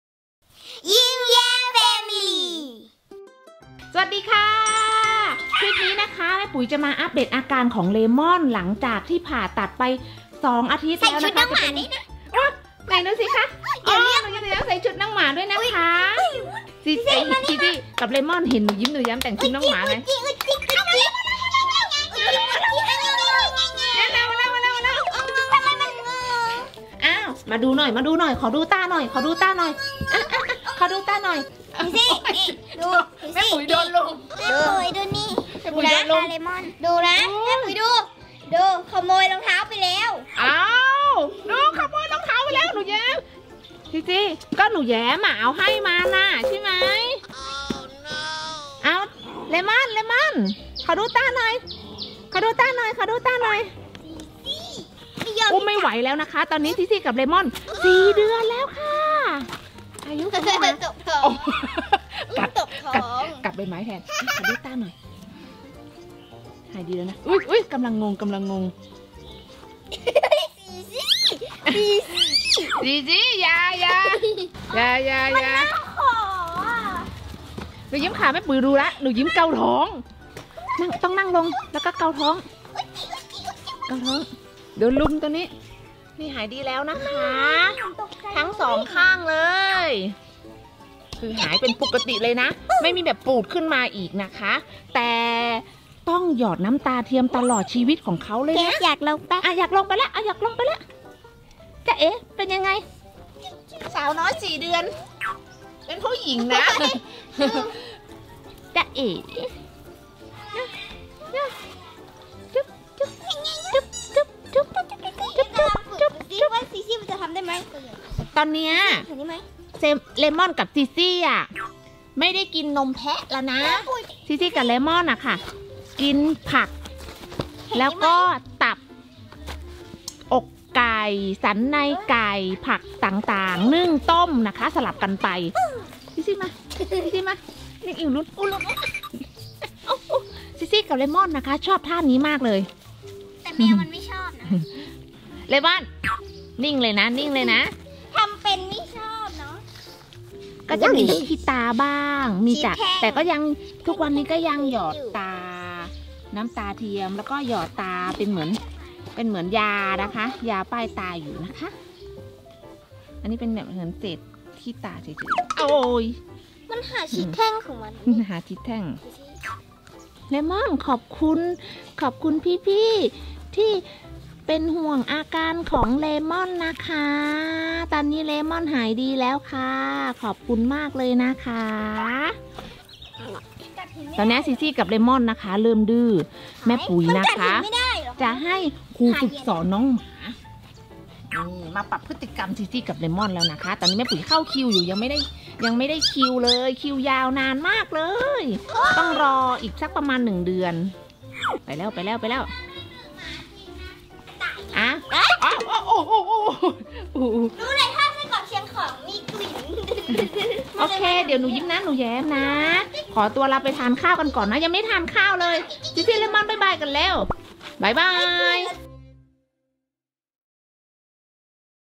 YimYamFamily | อัพเดทอาการเลม่อนหลังผ่าตัดตา
YimYamFamily บอกลาเชอร์รีอายของเลม่อนแล้วจ้า เลม่อนหายดีแล้ว ตาใสปิ๊งๆเลย เย้~